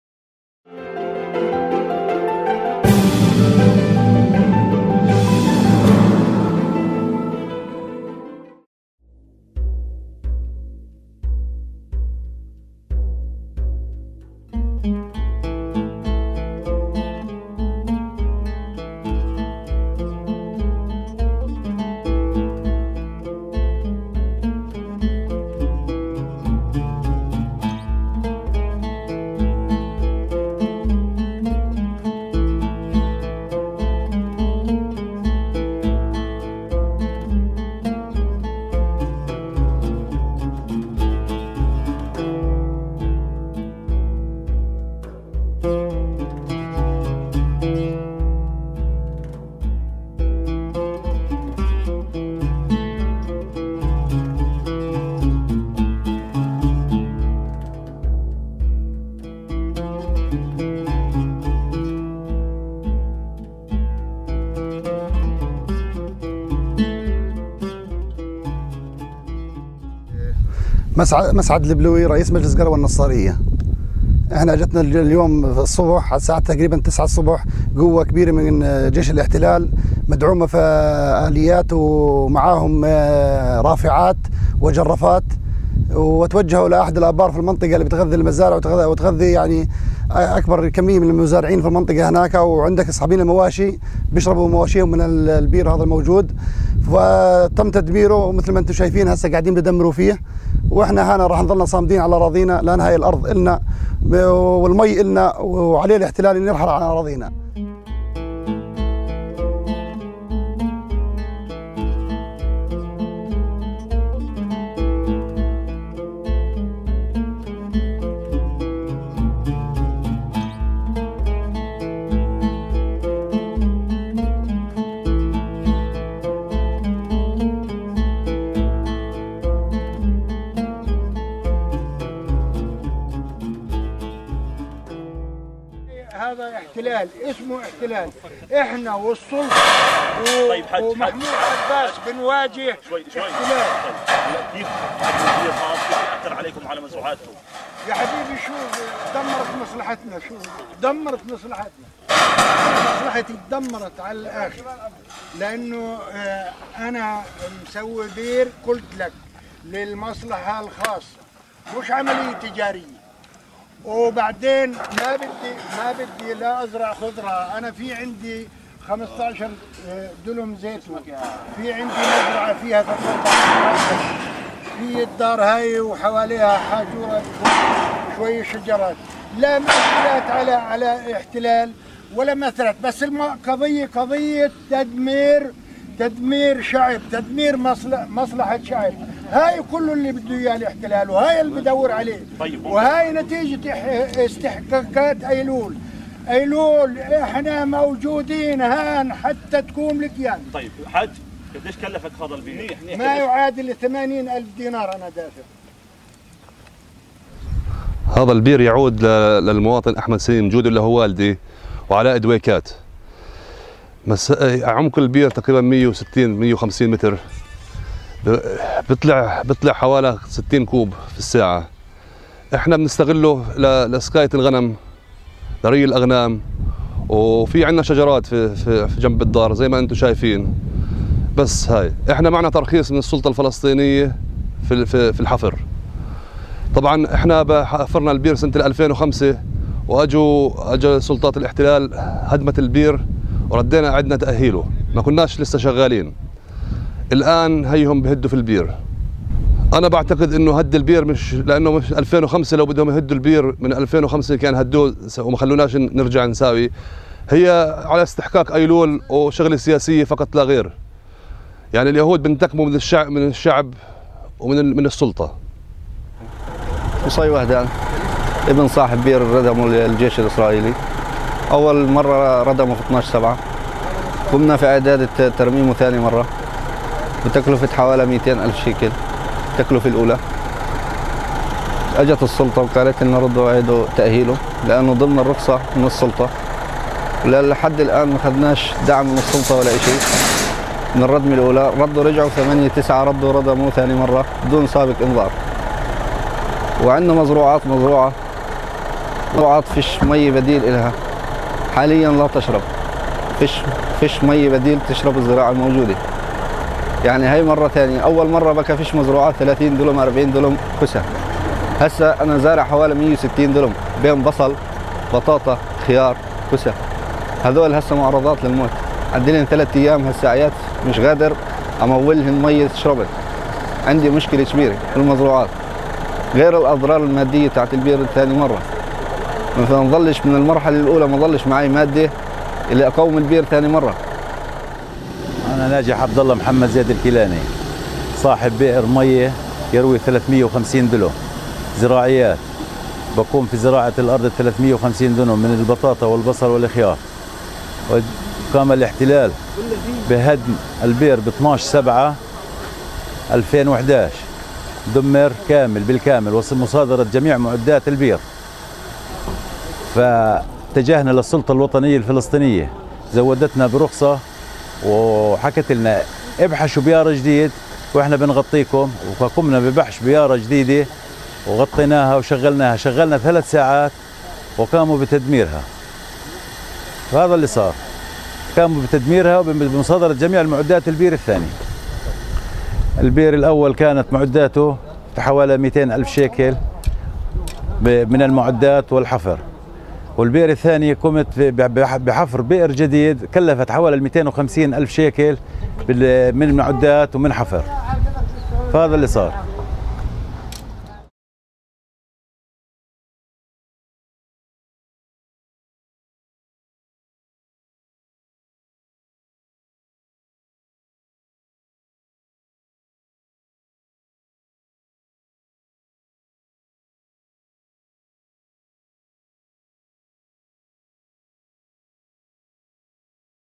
Demolshing of five artesian wells in Al-Nassariyya and Al-'Akrabaniyya wells
The Israeli Occupation Army bulldozers demolished of five artesian wells in Al-Nassariyya and Al-'Akrabaniyya communities in the Northern parts of the Jordan Valley. (September 13, 2011).